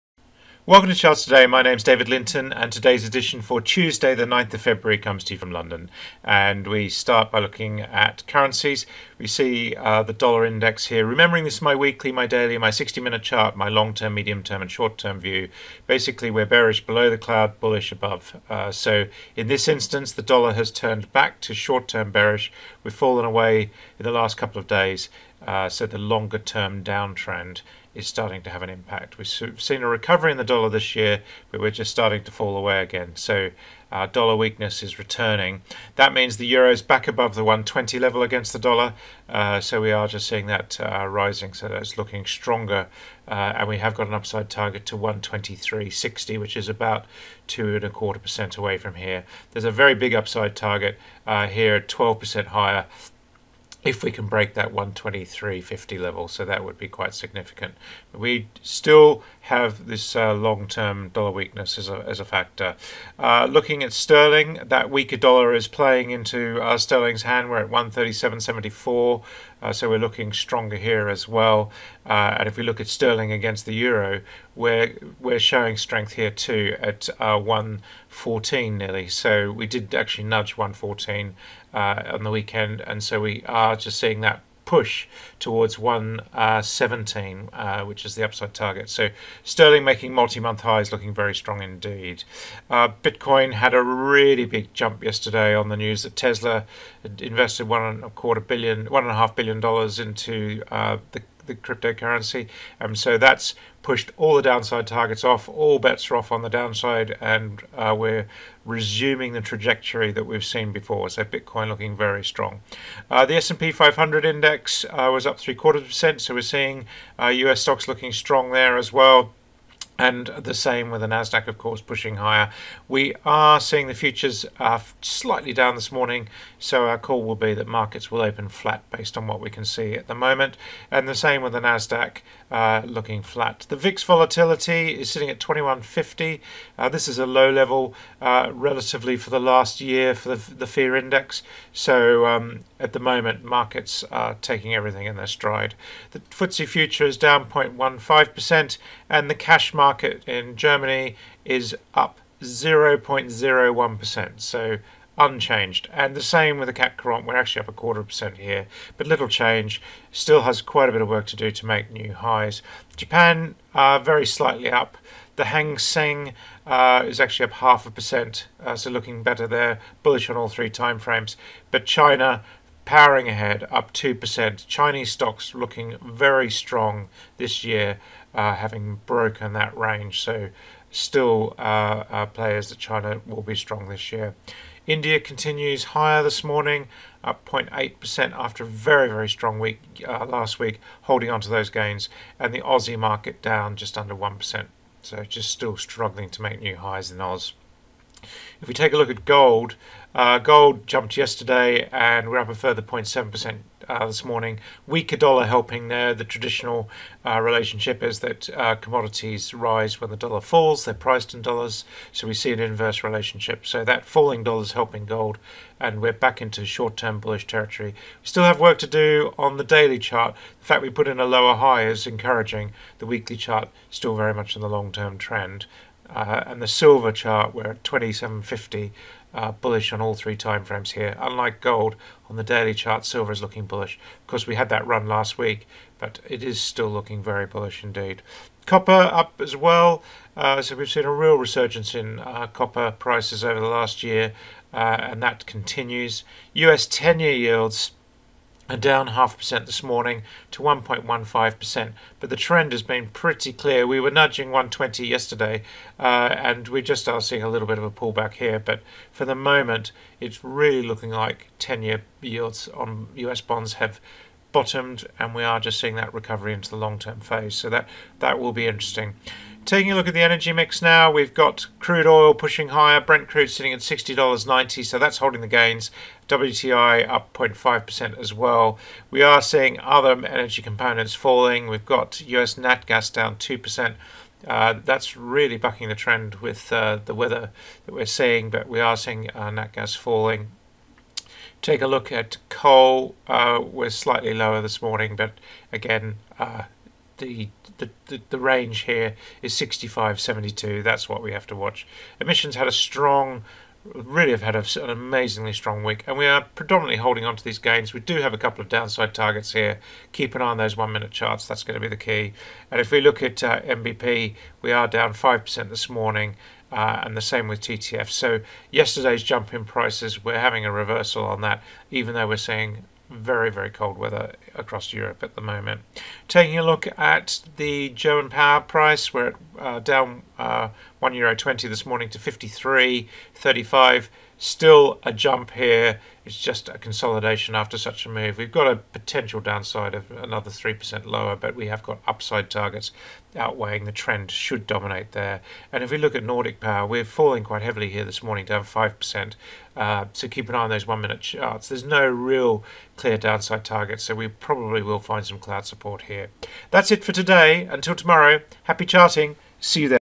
Charts Today – 9 Feb 2021 – Markets quiet but hold gains
Dollar weakens, Bitcoin new highs. US stocks set to open flat based on futures. Europe and Asia mostly flat. Gold and silver rise on weaker dollar. US Bond yields steady. Crude oil up, European pulls back.